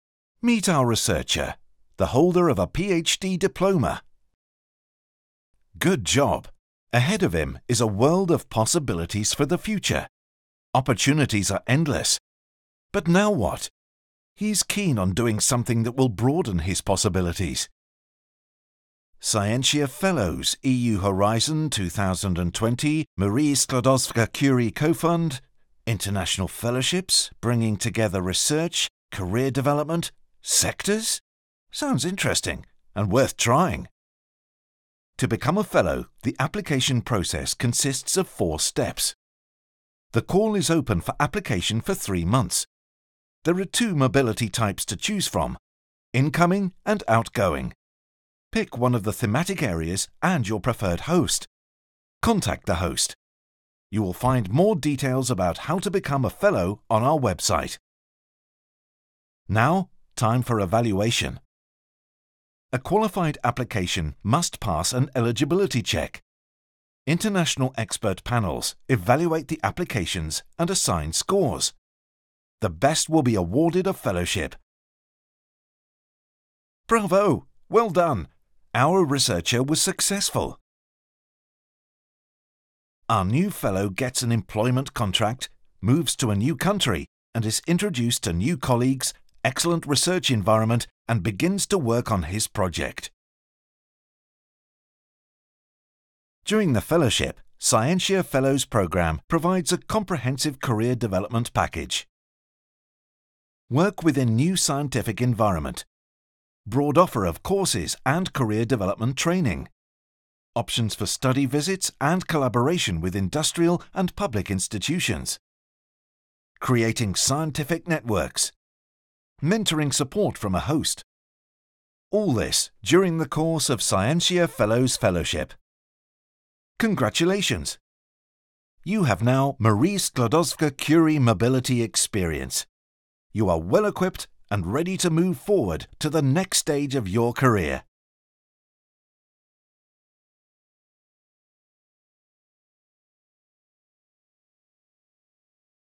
This is Scientia Fellows
Scientia Fellows is an international fellowship programme (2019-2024) in the field of Health Life Sciences. Through this programme, researchers can enhance their career, improve networking and develop new transferable skills. Key words are Excellent research, innovation activities and cross-sectoral collaborations involving academia, hospital and industry.

The programme receives funding from the European Union’s Horizon 2020 Research and Innovation programme under the Marie Skłodowska-Curie grant agreement No 801133.